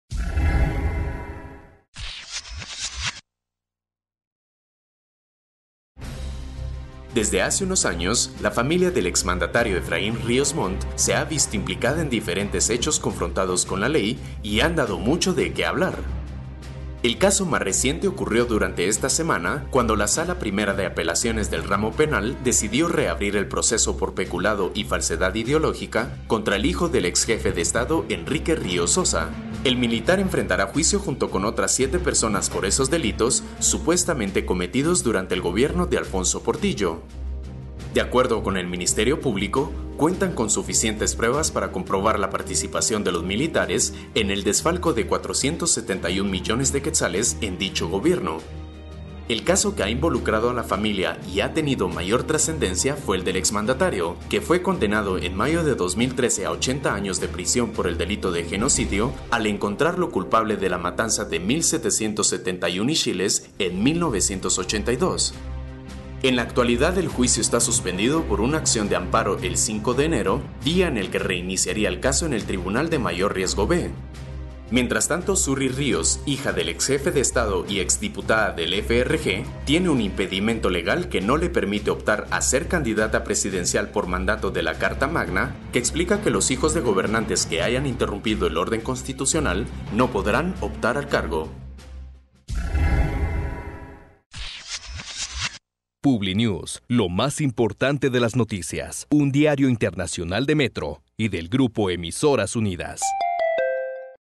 Una familia enfrentada con la ley
Desde hace unos años, la familia del exmandatario Efraín Ríos Montt se ha visto implicada en diferentes hechos confrontados con la ley y han dado mucho de qué hablar